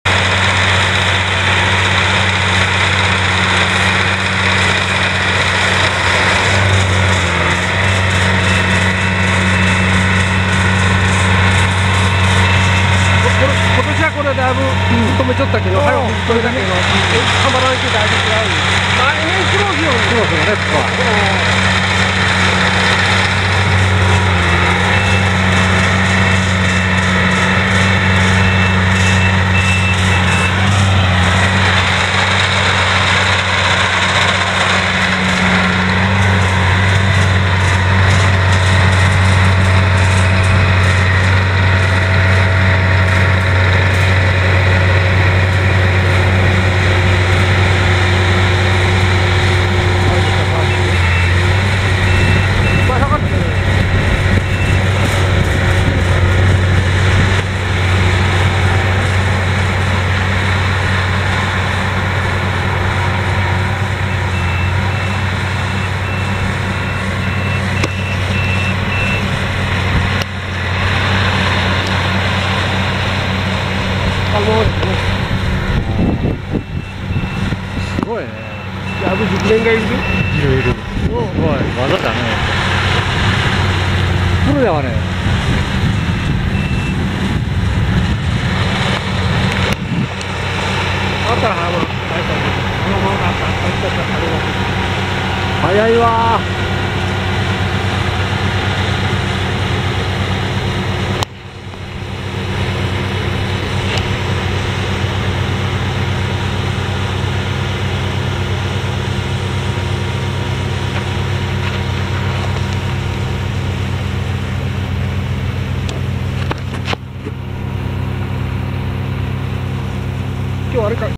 早い刈り取り ヤンマーコンバイン ＧＣ451
ヤンマーコンバイン　ＧＣ451
4条刈り
51馬力
自動水平機構付